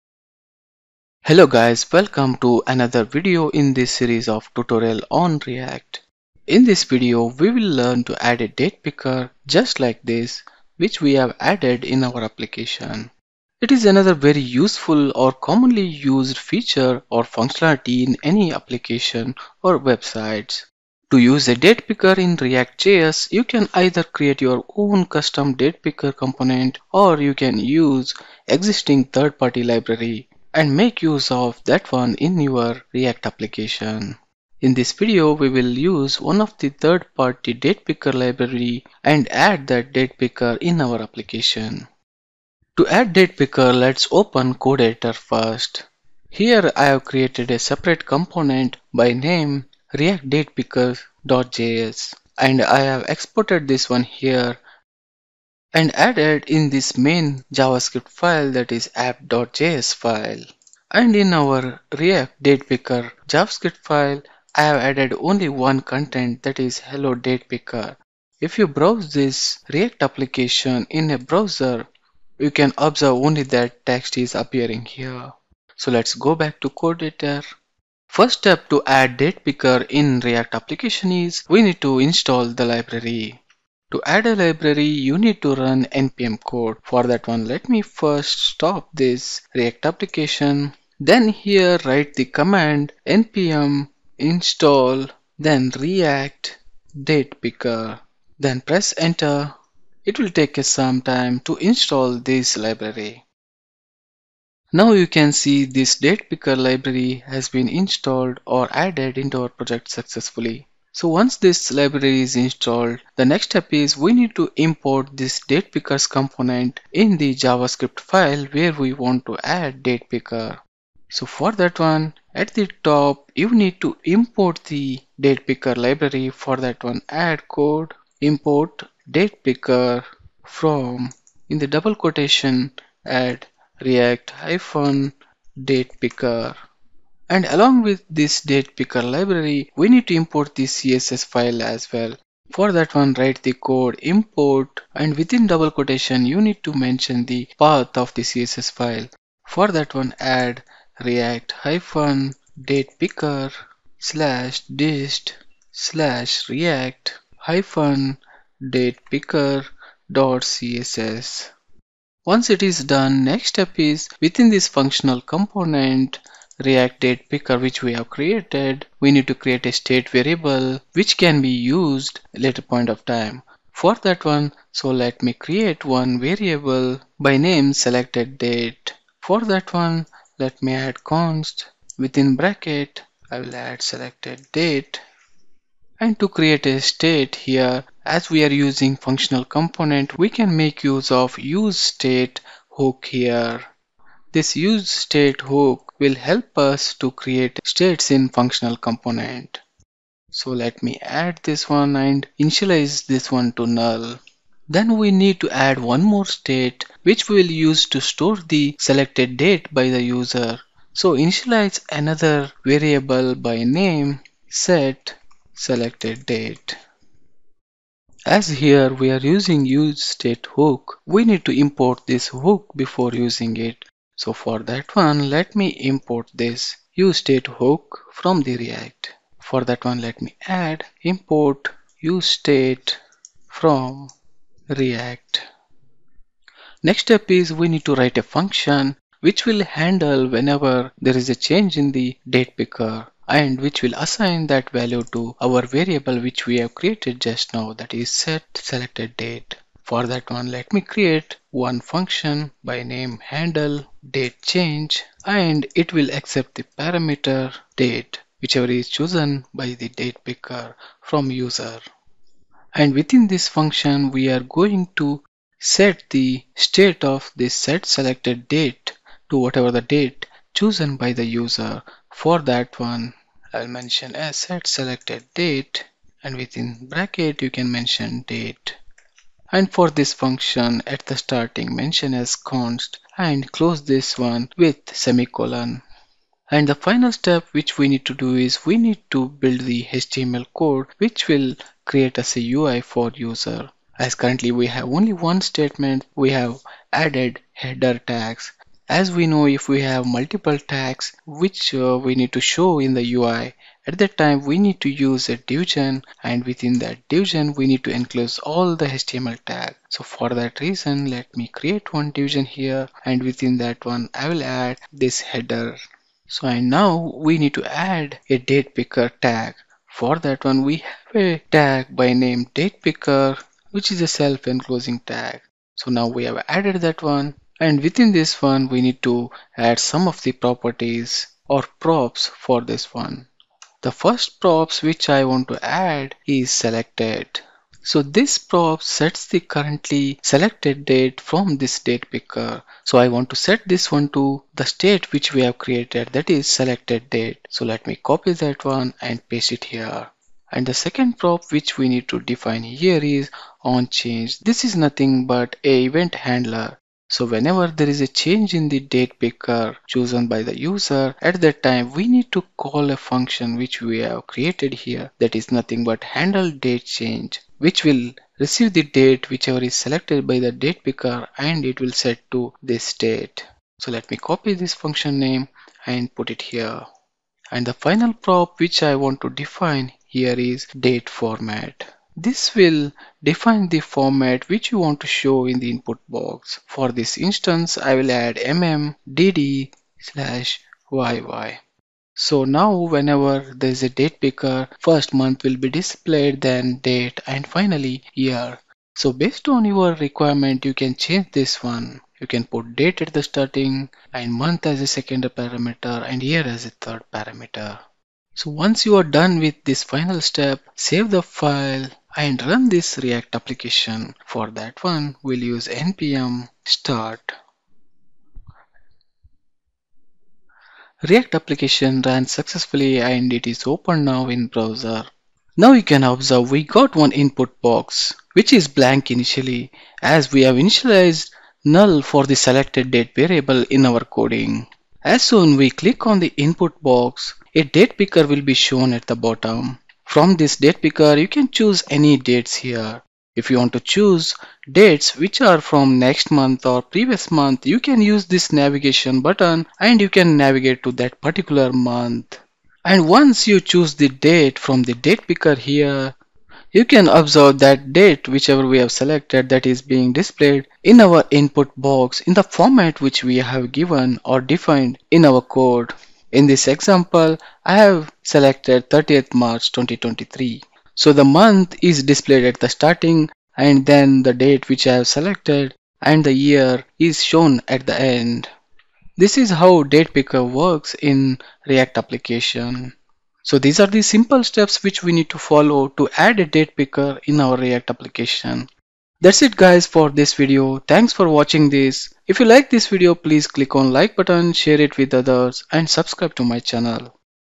ReactJS Projects: Adding Calendar / Date Picker in ReactJS Application
In this video, you will learn to add a one of the most useful feature / UI element - date picker in React Application.